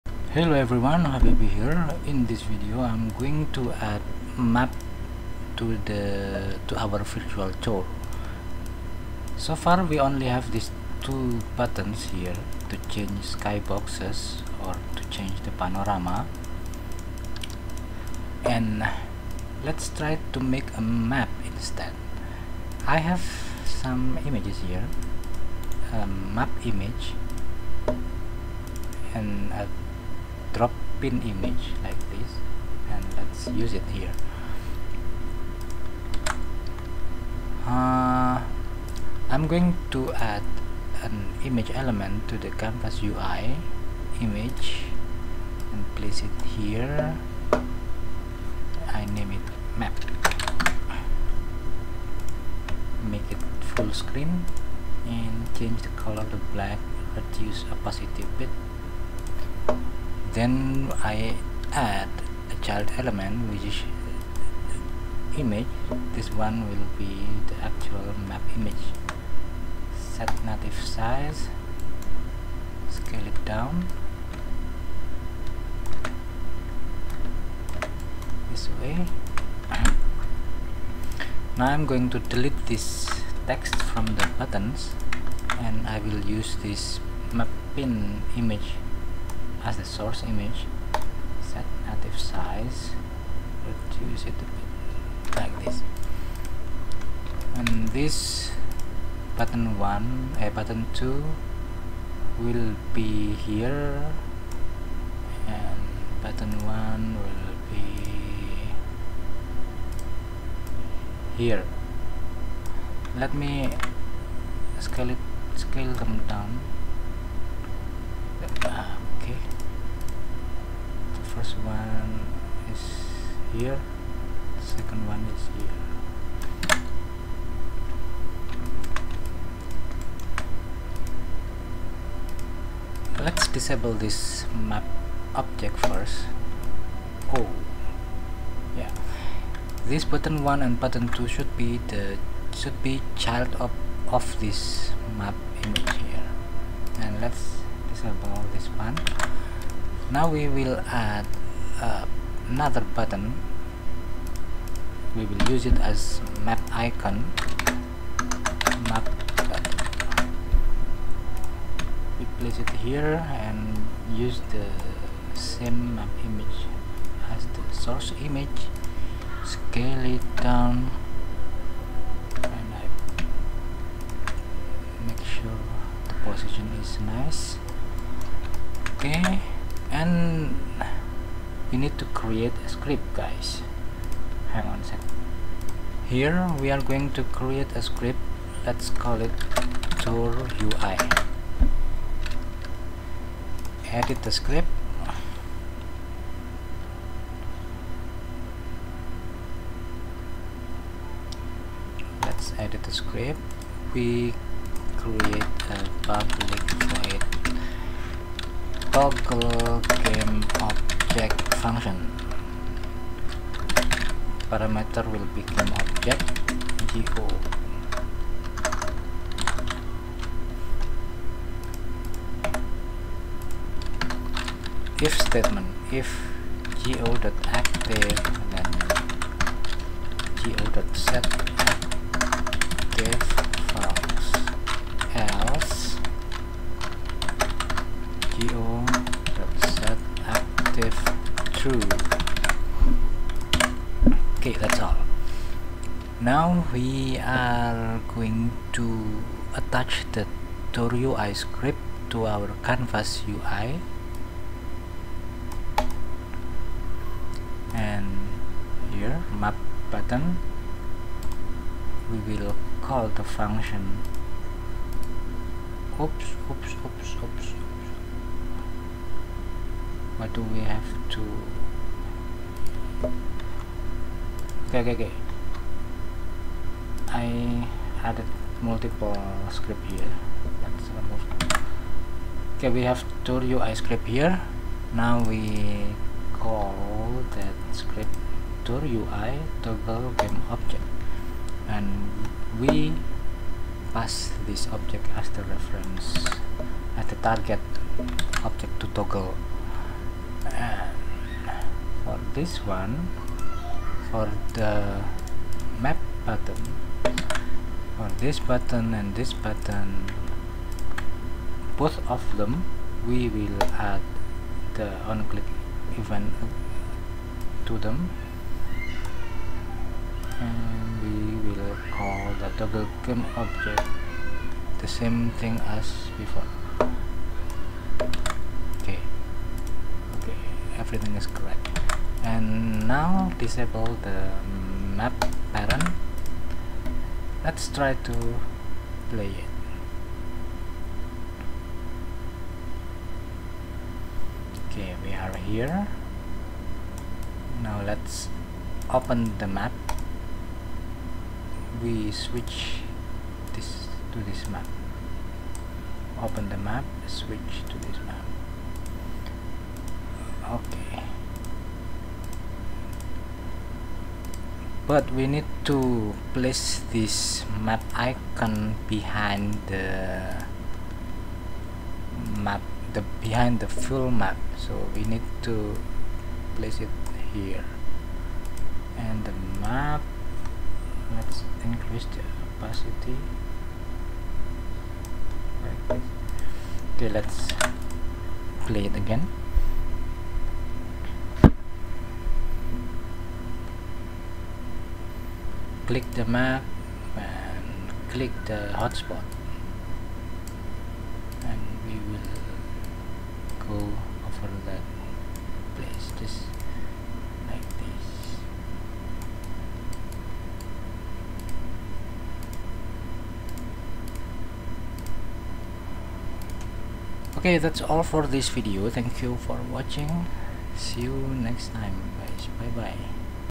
Adding map feature to our Unity virtual tour project
In this video I will show you how to add a simple and nice map system for our Unity 360 virtual tour project.

To watch the entire related videos about How to make 360 Panorama Virtual Tour in Unity, check out this tutorial playlist